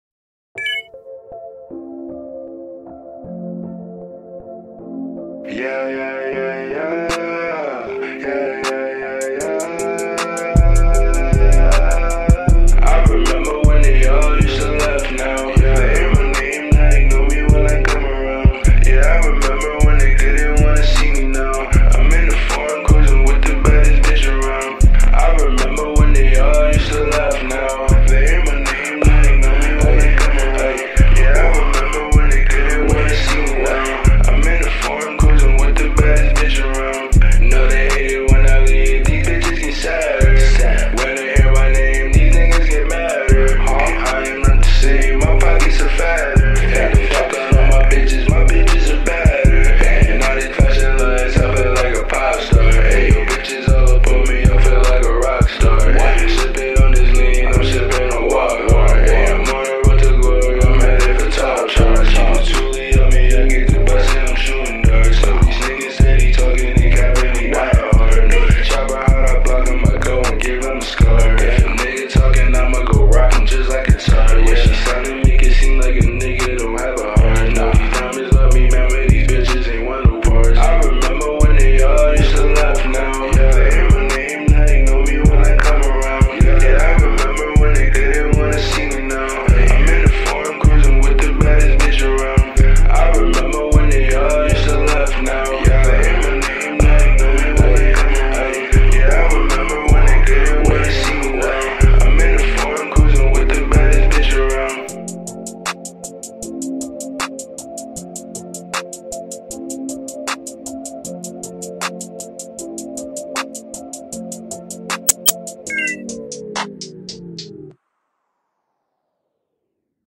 WE'LL MiSS U LiL PEEP
MY LiL TRiBUTE. i KNO, iTS NOTHiNG, BUT iTS MY "THANK YOU". WE MISS U GUS...